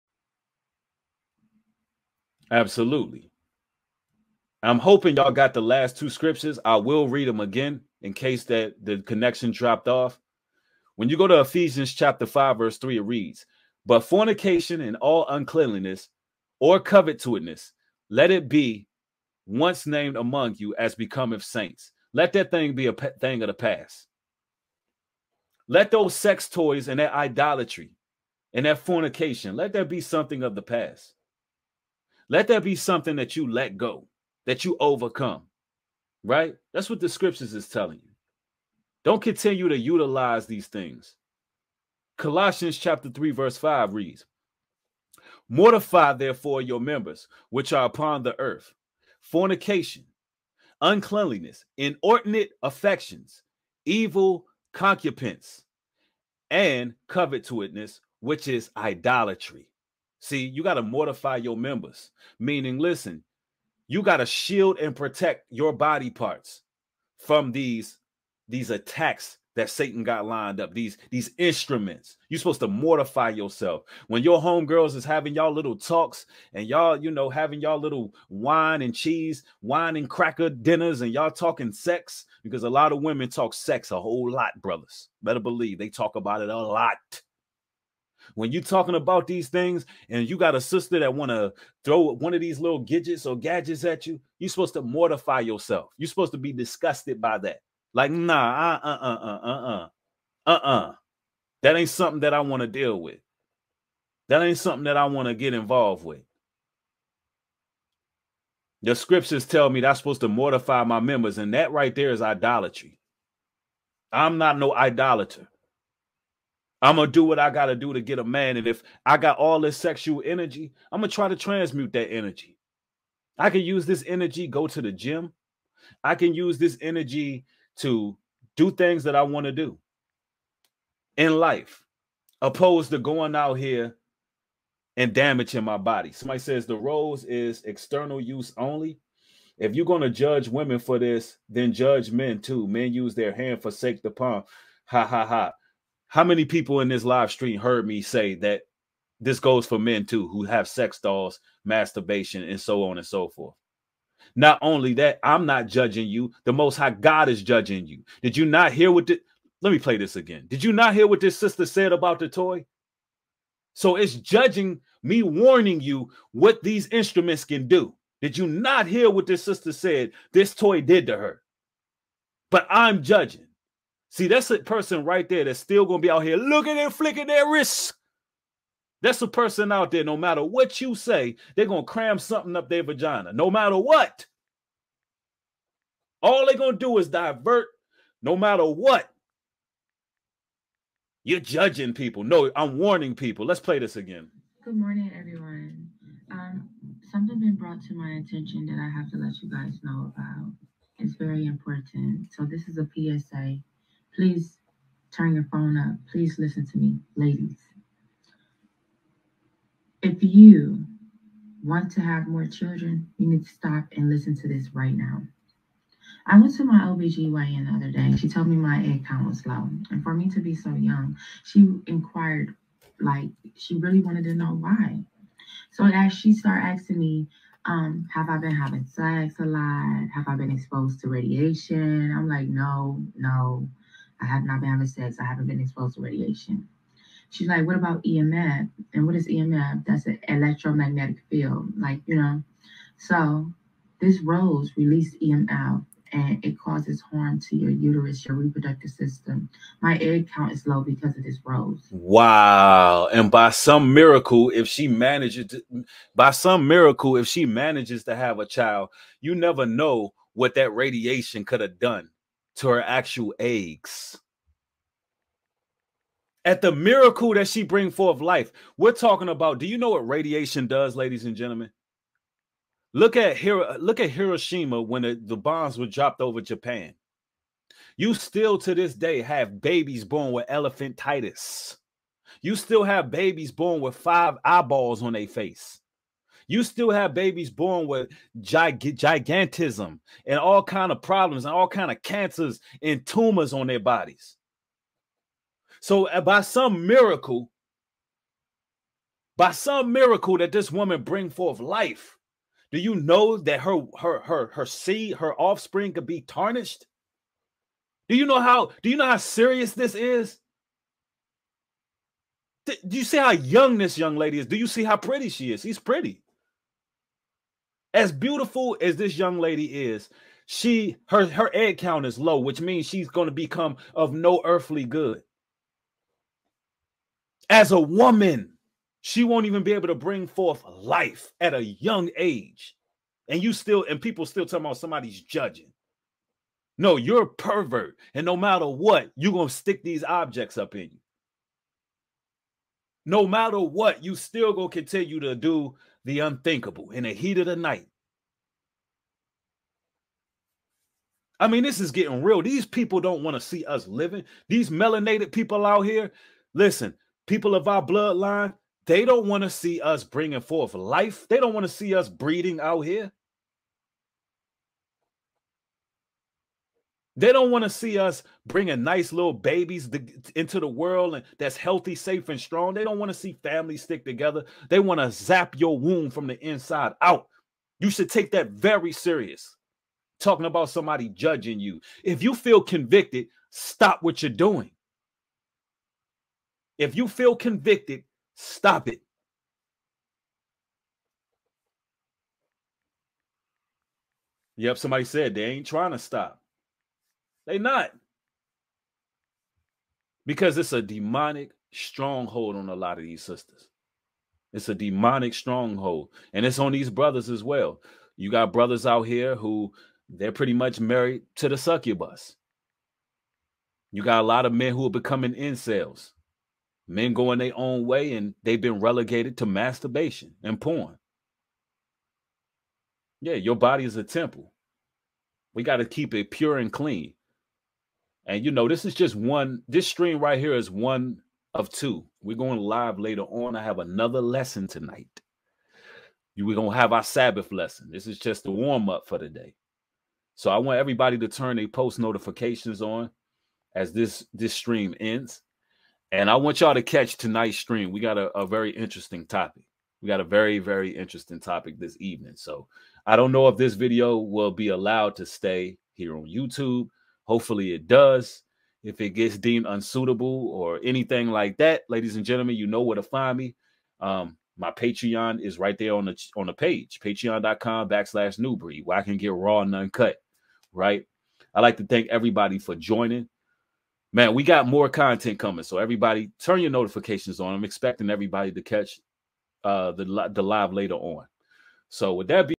TRUTH about the VIRAL rose🌹toy | Nubreed Global Truth
Ladies please pay attention and understand what is really going on out here. Let’s change.

A simple google search can tell you the LIST of chemicals and toxic plastics are used to make these “t o y s”. That should be enough to make you throw it out if you own one! I’m glad I never entertained these 😭💯